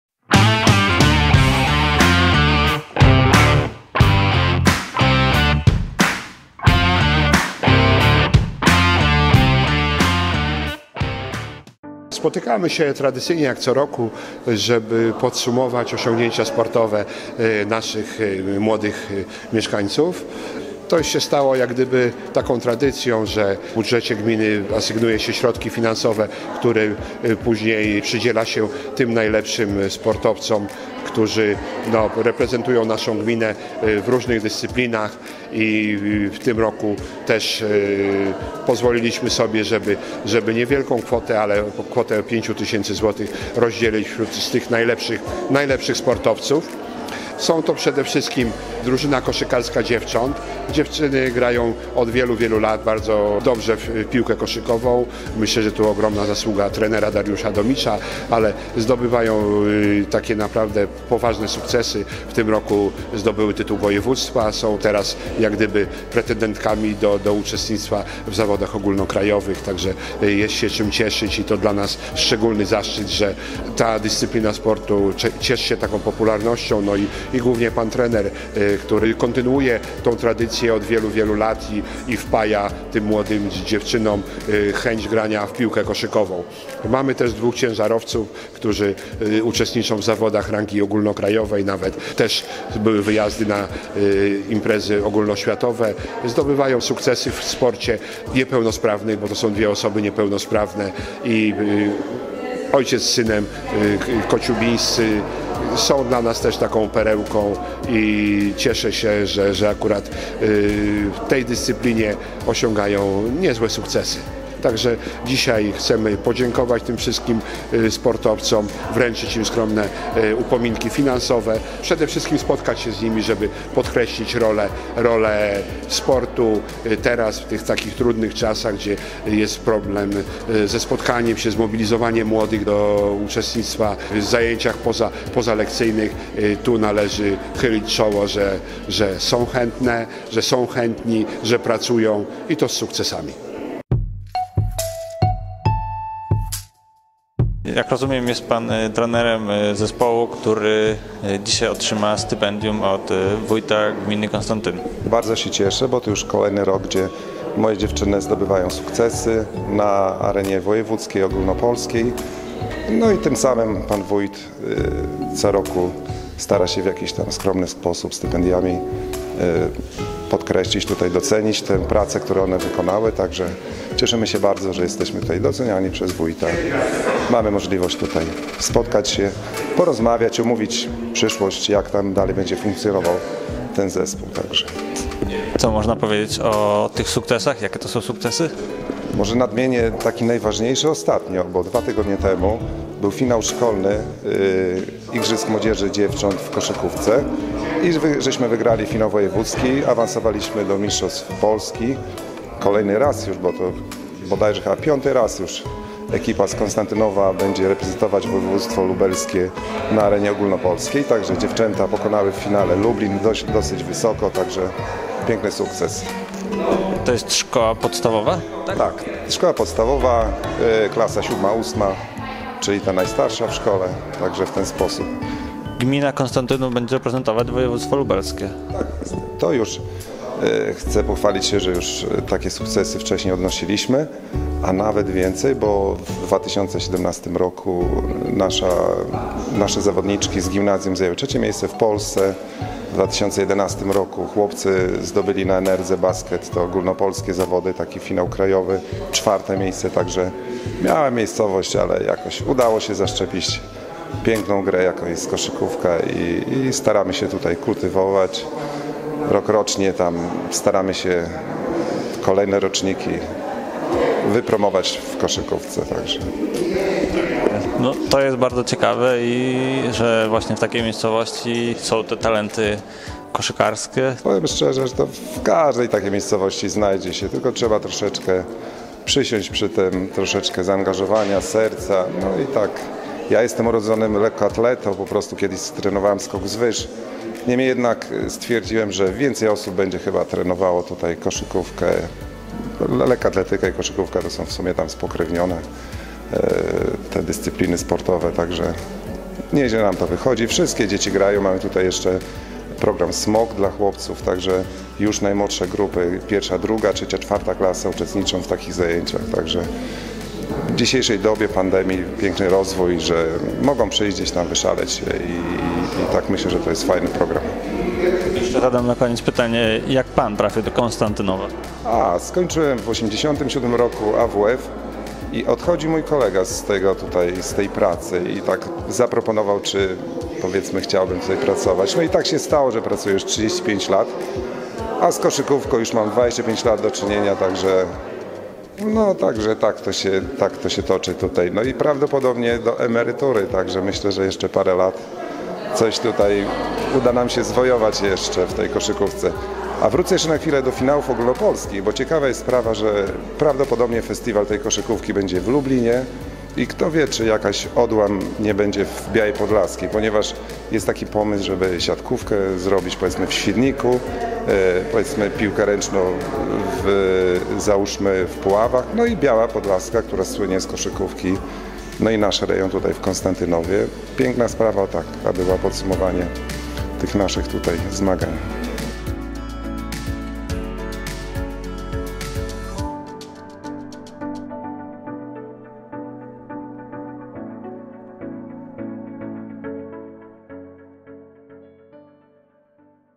Konstantynów: Wójt przyznał nagrody za osiągnięcia sportowe
5 kwietnia, w sali konferencyjnej Urzędu Gminy Konstantynów odbyła się uroczystość wręczenia nagród wójta gminy za osiągnięcia sportowe w 2021 roku. Nagrodę otrzymało łącznie 15 zawodników.